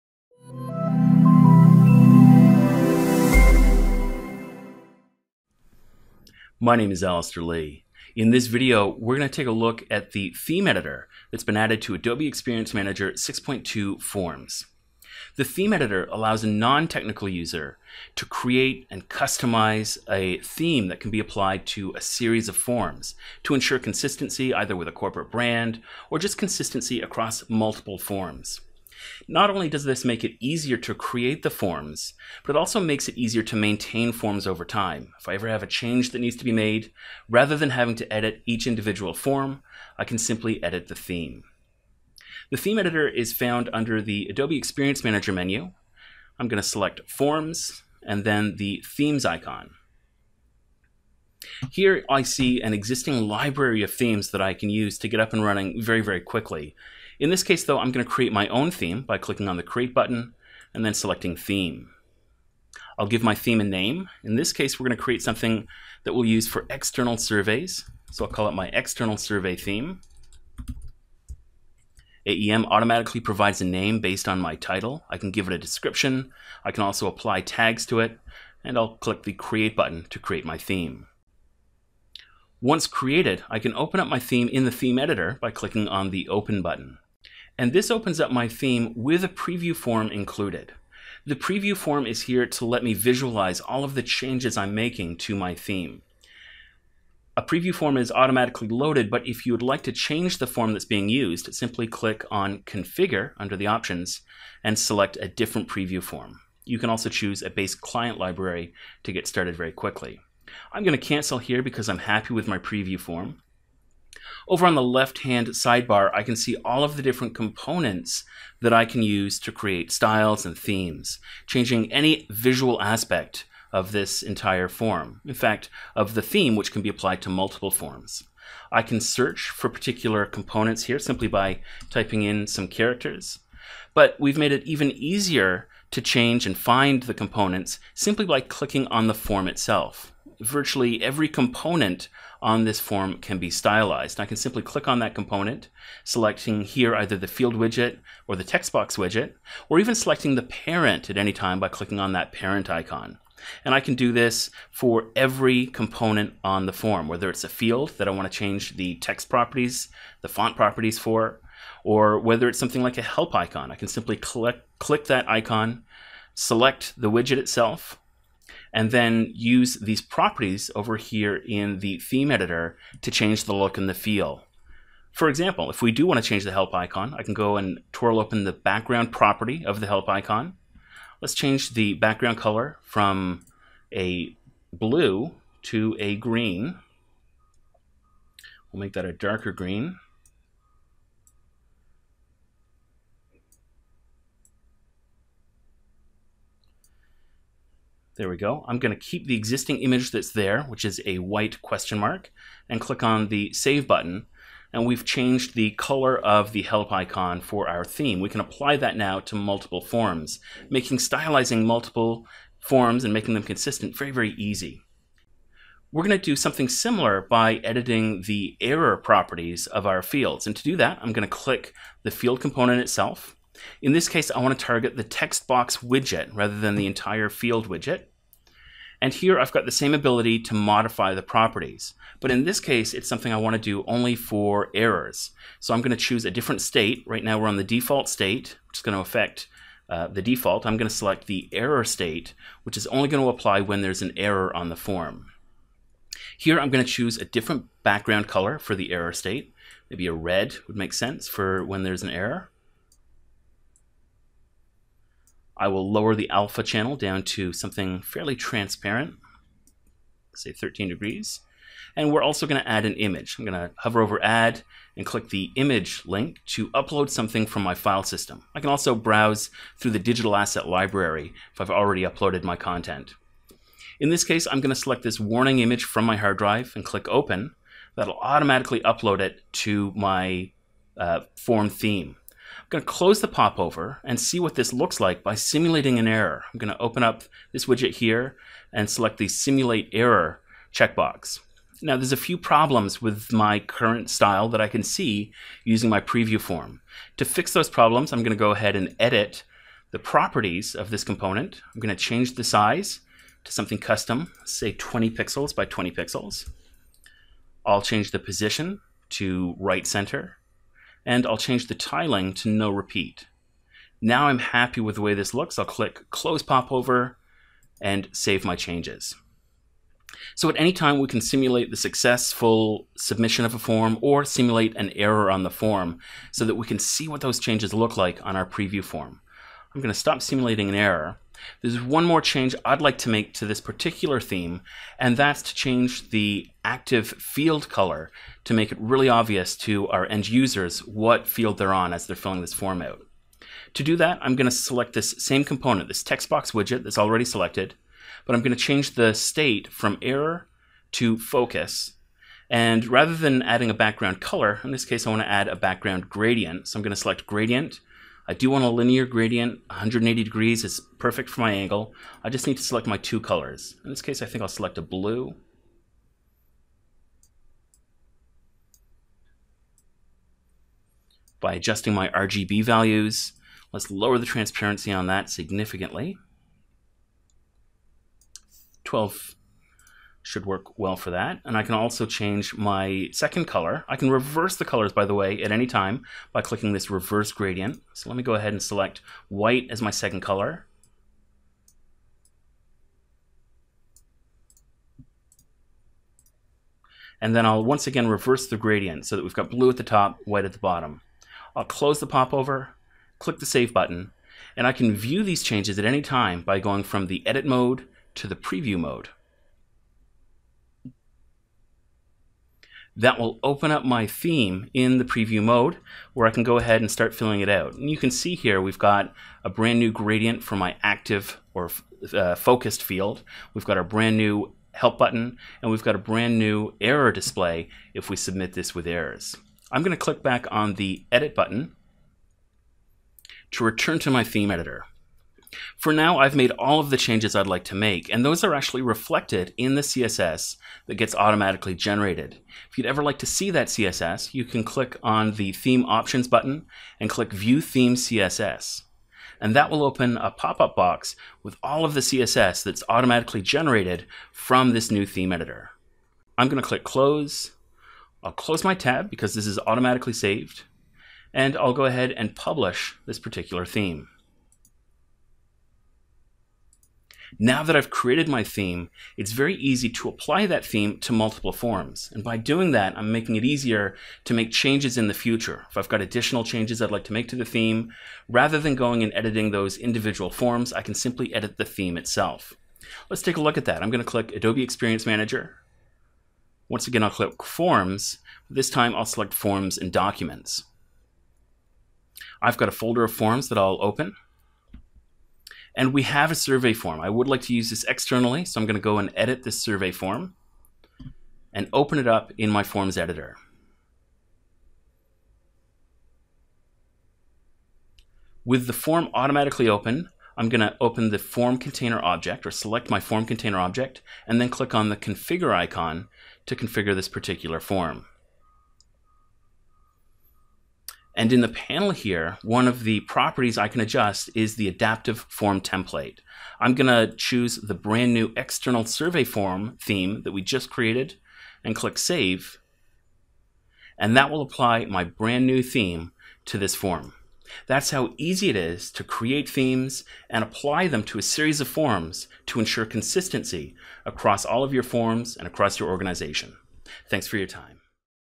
Using the Theme Editor with AEM Forms
This video illustrates how the theme editor can be used to ensure consistency across multiple forms and how it enables business users to stylize their forms without knowing CSS.